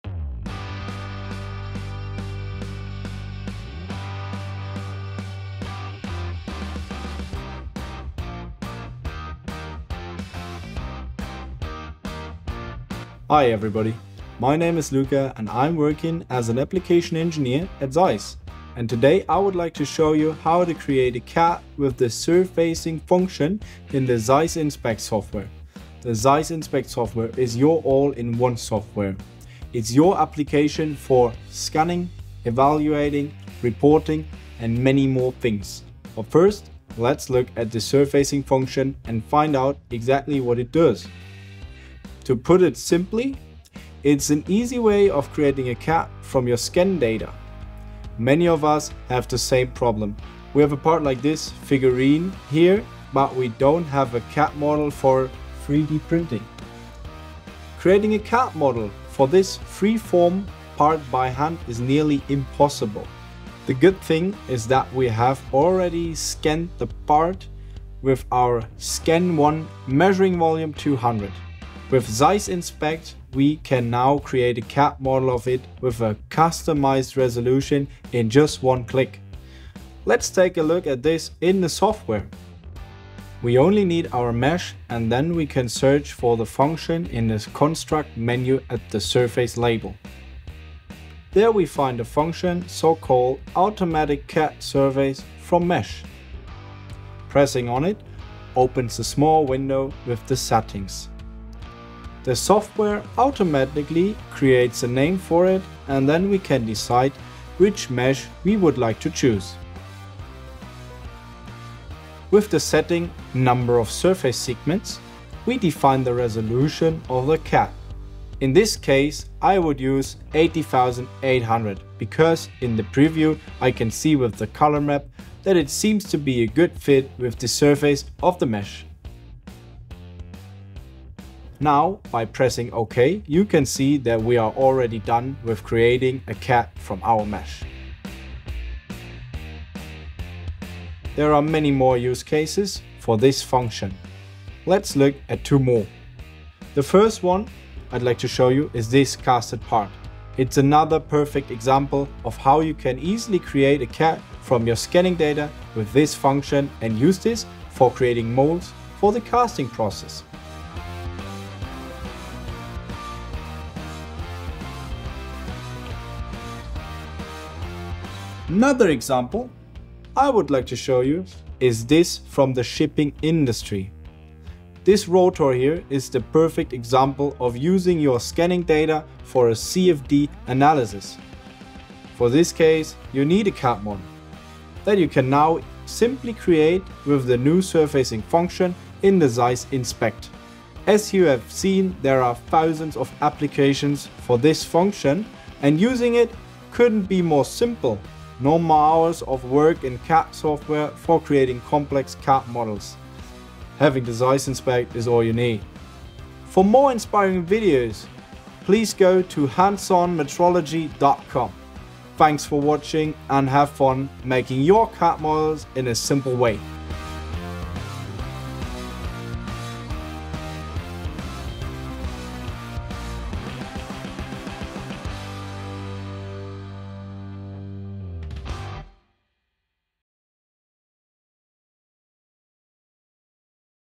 How to perform CAD surfacing in ZEISS INSPECT
No CAD available for projects like 3D printing? The new surfacing function in ZEISS INSPECT got you covered. With a simple mouse click you receive a customizable CAD based on our 3D scan data. Let Luca show you how to simplify the creation of CAD with the latest release ouf our software solution.

Do you have any questions about ZEISS INSPECT?